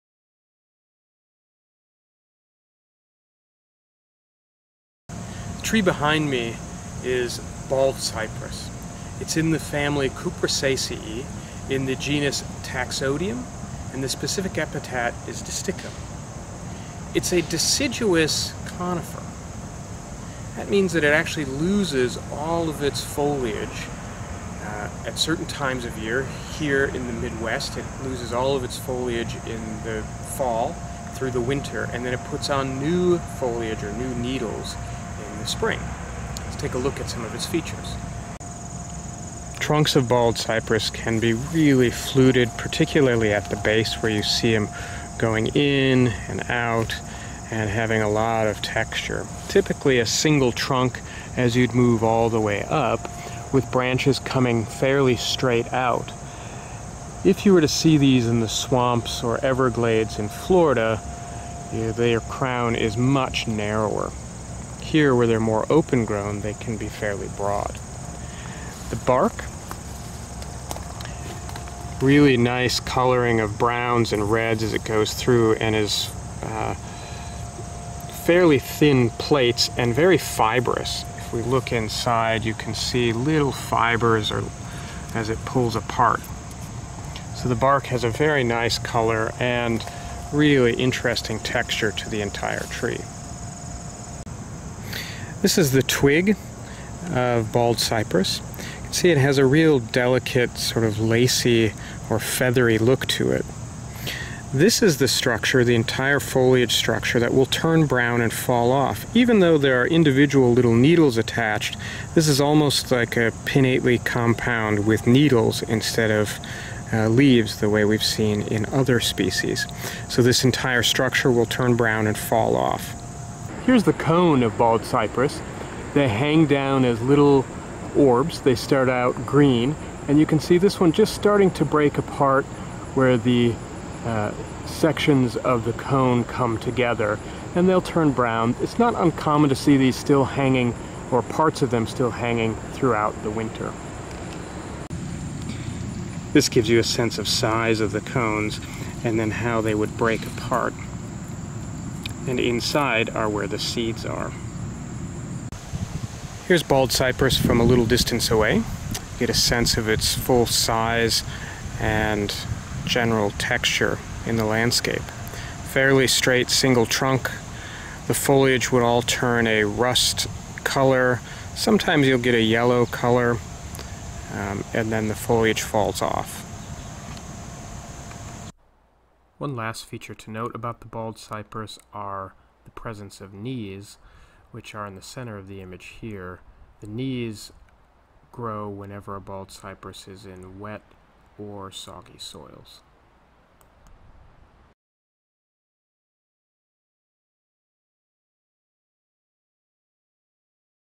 Identifying baldcypress
Identifying baldcypress - Taxodium distichum

Video created by to for the course NRES 201 - Dendrology to supplement class lectures and field walks.